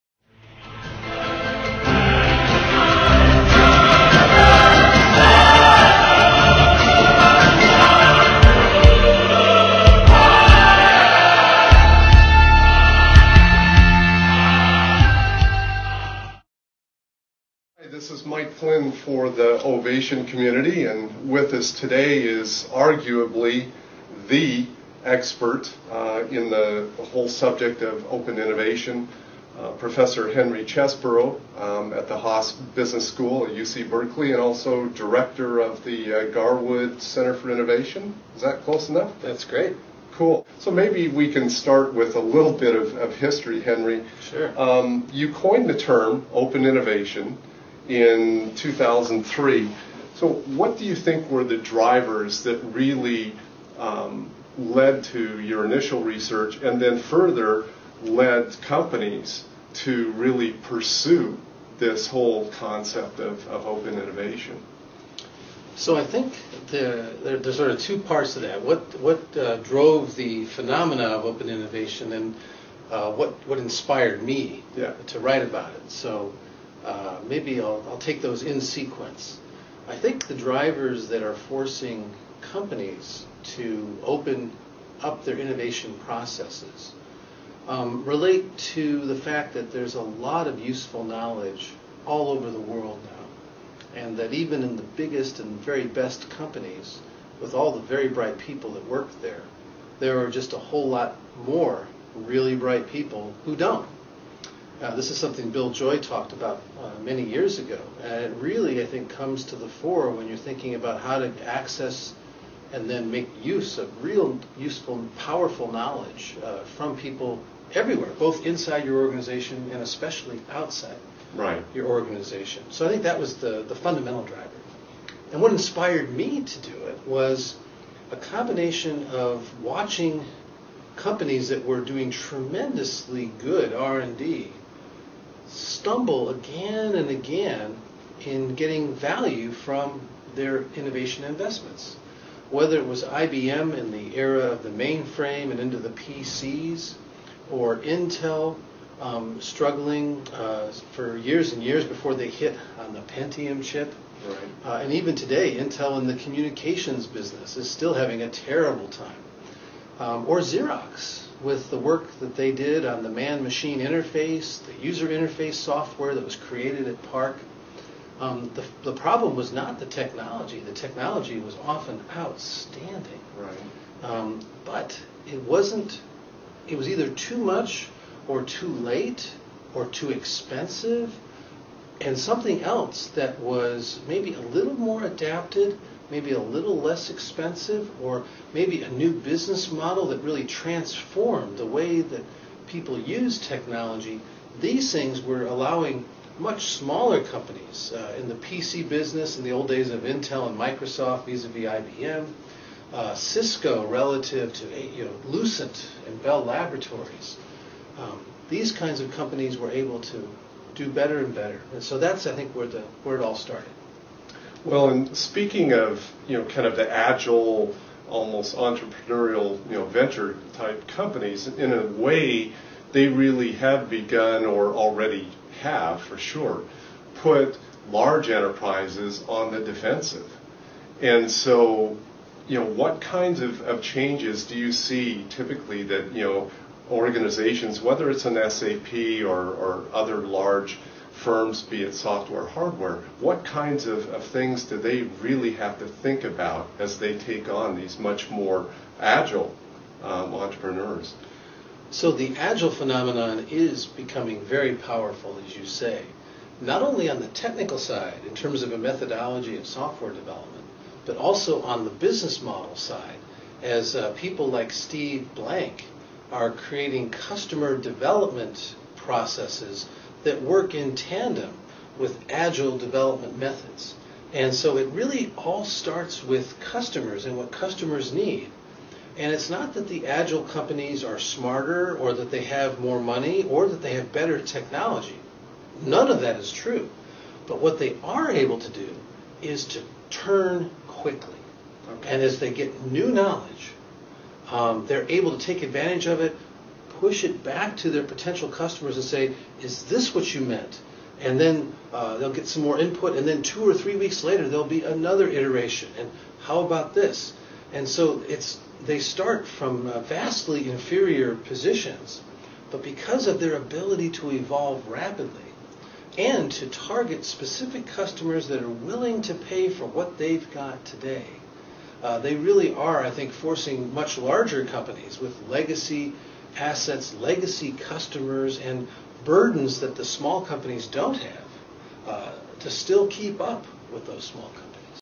Insights on Open Innovation with Henry Chesbrough - Part 1 (Introduction & Motivations).mp4
This is part 1 of a series on Open Innovation concepts from the father of Open Innovation, Professor Henry Chesbrough. Produced by SAP's Technology Innovation and Developer Experience team. In this installment, Professor Chesbrough presents an introduction and motivations for the practice of Open Innovation.